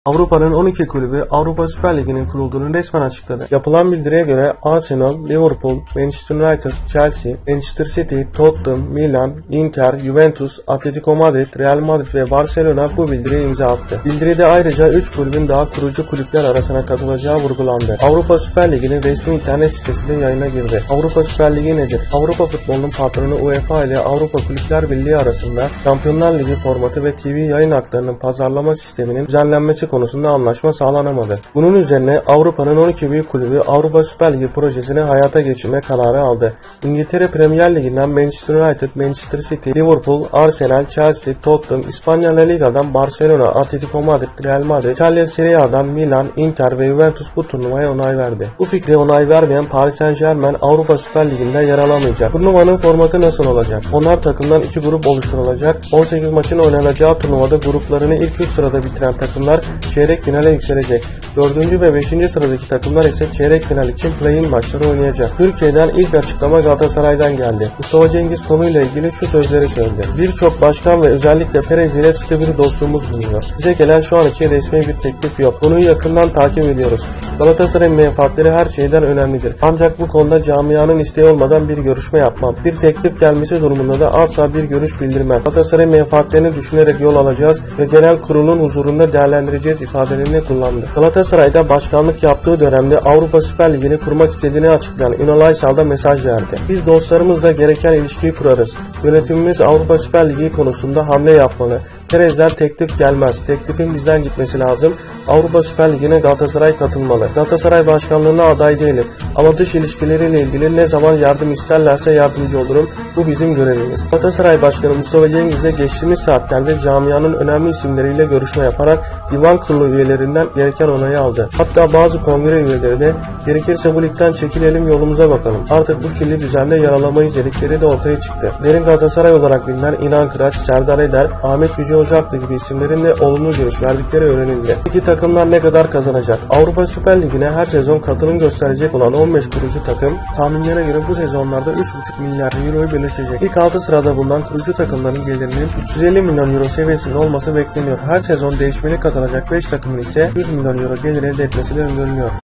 DERİN GALATASARAY AVRUPA SÜPER LİGİ İÇİN DEVREDE
Avrupa Süper Ligi'ne katılım için Türk takımlarının tutumu çok merak ediliyordu. Galatasaray kulübünden ilk adımlar geldi...

KURUCU ÜYELER KİMLER?

Premier Lig'den Manchester United, Manchester City, Liverpool, Chelsea, Arsenal ve Tottenham, Serie A'dan Juventus, Milan ve Inter, La Liga'dan ise Real Madrid, Barcelona ve Atletico Madrid yeni kurulan bu lige kurucu üye olarak katılacaklarını açıkladı

TURNUVAYI KİM FİNANSE EDECEK?

ABD'li bankacılık grubu JPMorgan Chase Avrupa Süper Ligi'ni finanse eden kuruluş olarak öne çıkıyor. Proje için 6 milyar dolarlık kaynak oluşturduğu tahmin edilen JPMorgan Chase futbolun NBA'ini kurmayı planlıyor.

Etiketler: Galatasaray Fenerbahçe Beşiktaş futbol Florentino Perez.